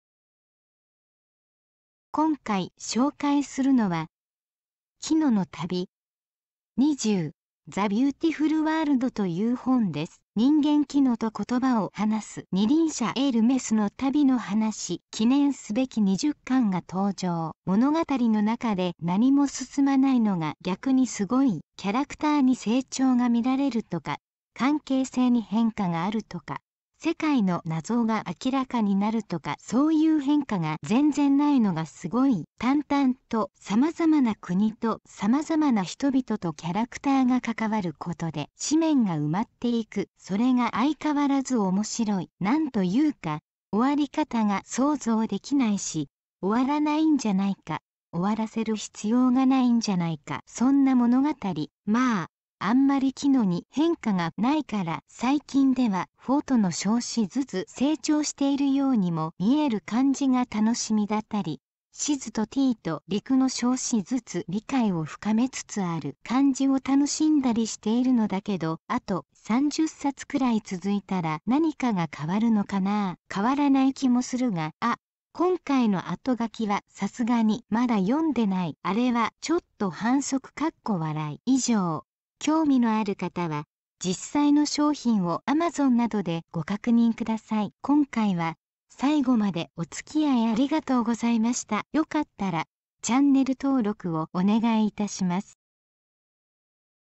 【紹介】キノの旅 20 the Beautiful World 電撃文庫 （時雨沢 恵一,黒星 紅白）
今回紹介するのは、　キノの旅 20 the Beautiful World　という本です。

人間キノと言葉を話す二輪車エルメスの旅の話。記念すべき20巻が登場!

物語の中で何も進まないのが逆に凄い。
キャラクターに成長が見られるとか、関係性に変化があるとか、世界の謎が明らかになるとかそういう変化が全然無いのが凄い。
淡々と様々な国と様々な人々とキャラクターが関わることで紙面が埋まって行く。それが相変わらず面白い。なんというか、終わり方が想像出来ないし、終わらないんじゃないか、終わらせる必要がないんじゃないか。そんな物語。
まぁ、あんまりキノに変化が無いから最近ではフォトの少しづつ成長しているようにも見える感じが楽しみだったり、シズとティーと陸の少しづつ理解を深めつつある感じを楽しんだりしているのだけど。
あと30冊くらい続いたら何かが変わるのかなぁ。変わらない気もするが。
あ、今回の後書きはさすがにまだ読んでない。あれはちょっと反則(笑)。